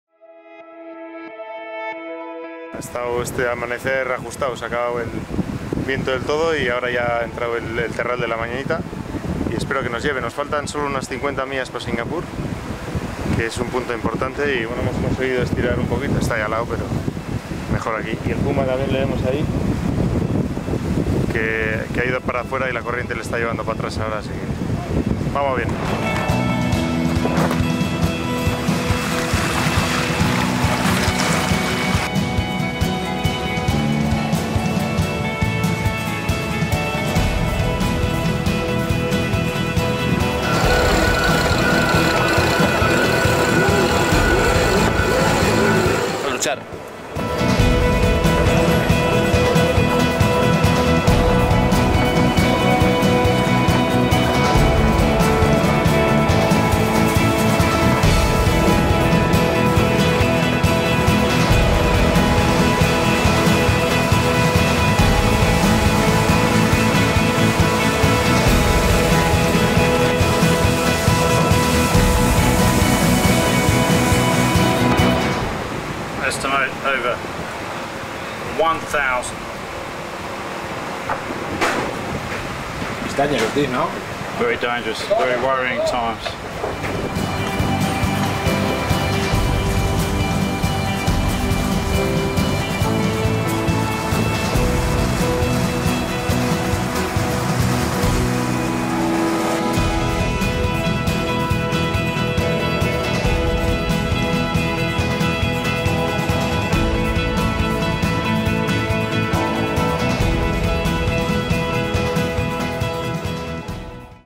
El "Telefónica" defiende su liderato en las últimas millas del estrecho de Malaca
Tras más de 72 intensas horas de navegación desde que el "Telefónica" superaba el punto que marcaba la entrada en el estrecho de Malaca, Palau We, el barco español se aproxima a Singapur defendiendo el liderato en una de las zonas más peligrosas para la navegación en la Volvo Ocean Race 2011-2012 por el denso tráfico de mercantes.